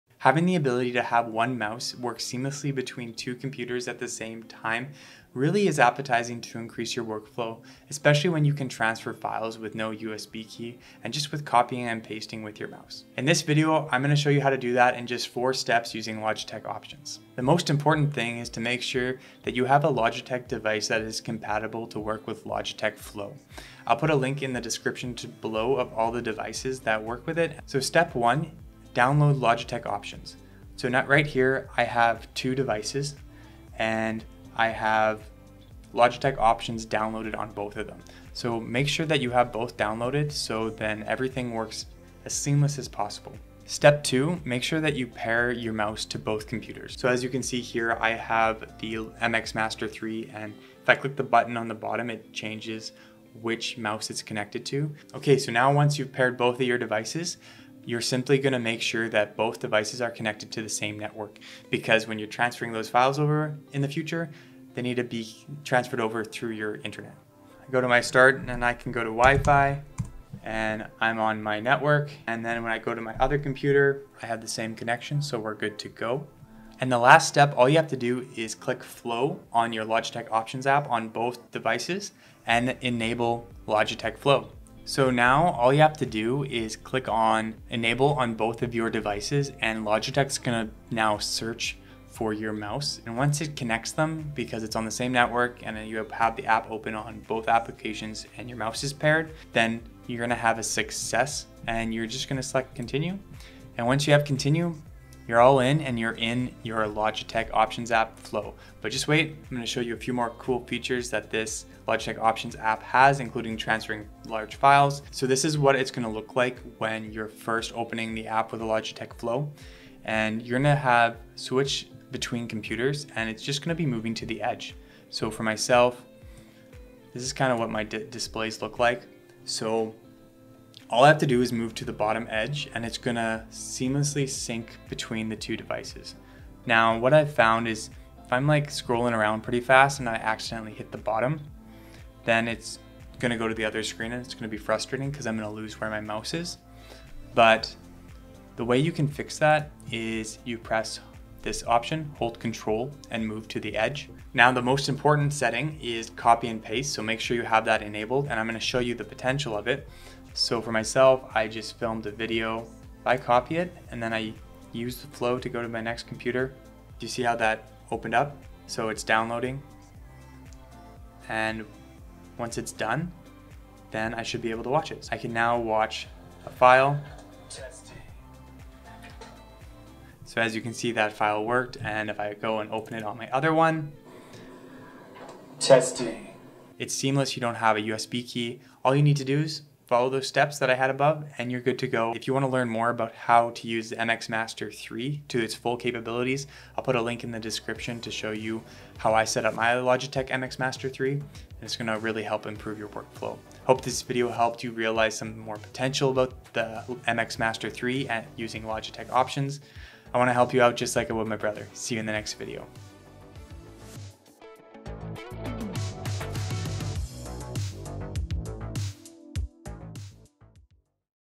Logitech Flow // Fast Setup!
If you want to use one mouse for two computers you are in the right place. I will show you how to use Logitech Options so you can set up Logitech Flow. Once you are able to set this up you will now be able to do some very cool stuff like copying and pasting large files with no USB!

MX Master 3
MX Master 2
MX Ergo
MX Anywhere 2S
M585 Multi-Device SIlent
Multi-Device M590

0:00 - Intro
0:27 - Download Logitech Options
0:55 - Connect To The Same Network
1:16 - Select Flow On Logitech Options
1:55 - Feature #1
2:34 - Copy & Paste Feature #2